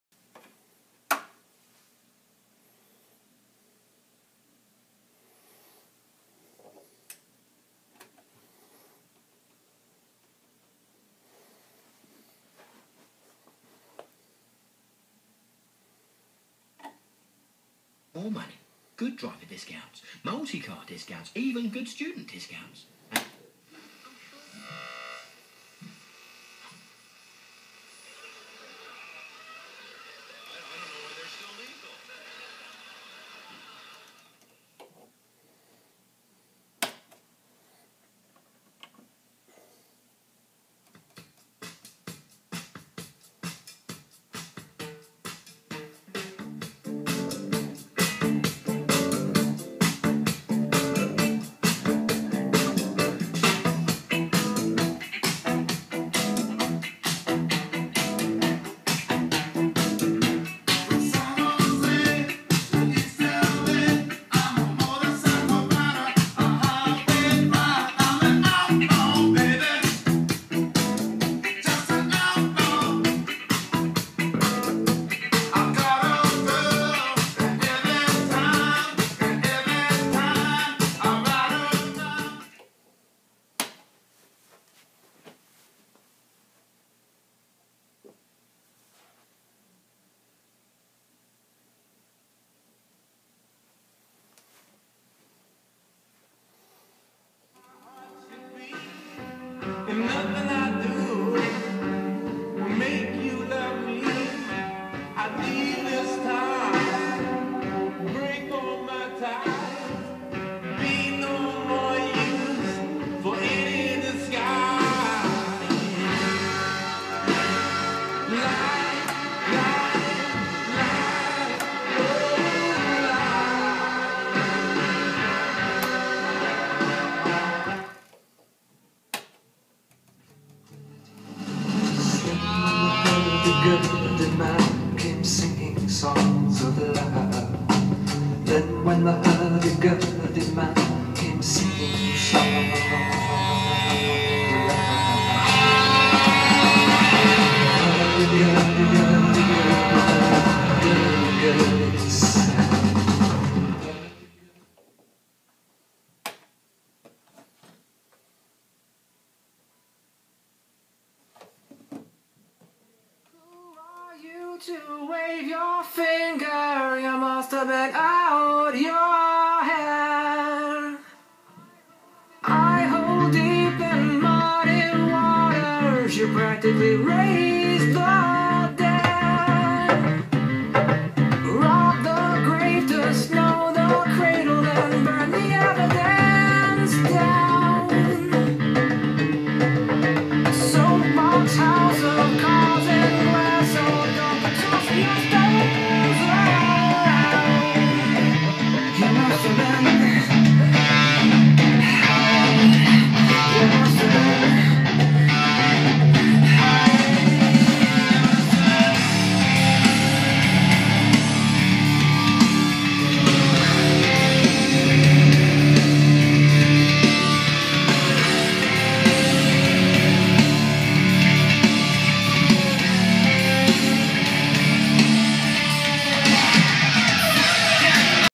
Pioneer SX1080 Demo
Demo with JBL L100 speaker, Dual 1019, Dual 1229q, and a Luxman D103u compact disk player.